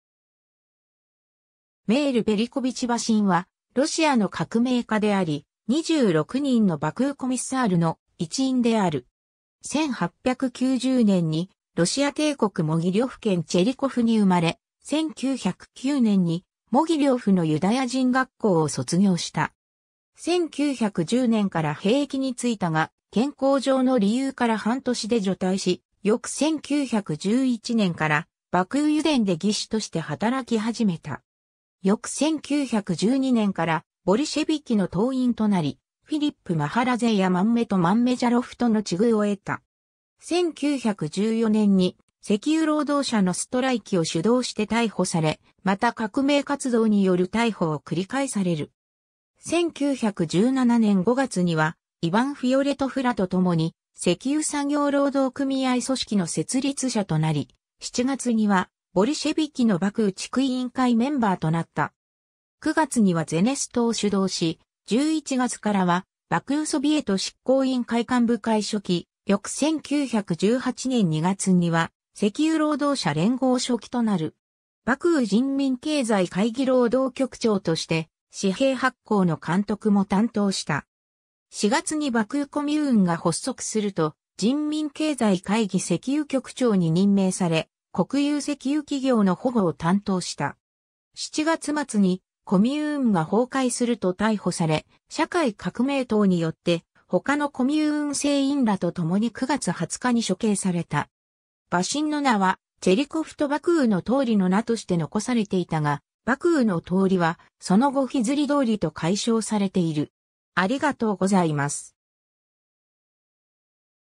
メール・バシン
バクー・コミューンの人物
メール（メイル）・ヴェリコヴィチ・バシン（ロシア語: Меер (Меир) Велькович Басин、1890年 - 1918年9月20日）は、ロシアの革命家であり、26人のバクー・コミッサールの一員である。
 1890年にロシア帝国モギリョフ県チェリコフに生まれ、1909年にモギリョフのユダヤ人学校を卒業した。
1910年から兵役に就いたが、健康上の理由から半年で除隊し、翌1911年からバクー油田で技師として働き始めた。
翌1912年からボリシェヴィキの党員となり、フィリップ・マハラゼやマンメト・マンメジャロフ (az) との知遇を得た。
 1914年に石油労働者のストライキを主導して逮捕され、また革命活動による逮捕を繰り返される。
1917年5月にはイヴァン・フィオレトフらとともに石油産業労働組合組織の設立者となり、7月にはボリシェヴィキのバクー地区委員会メンバーとなった。
9月にはゼネストを主導し、11月からはバクー・ソビエト執行委員会幹部会書記、翌1918年2月には石油労働者連合書記となる。
バクー人民経済会議労働局長として、紙幣発行の監督も担当した。
4月にバクー・コミューンが発足すると、人民経済会議石油局長に任命され、国有石油企業の保護を担当した。
 7月末にコミューンが崩壊すると逮捕され、社会革命党によって他のコミューン成員らとともに9月20日に処刑された。
バシンの名はチェリコフとバクーの通りの名として残されていたが、バクーの通りはその後フィズリ通りと改称されている。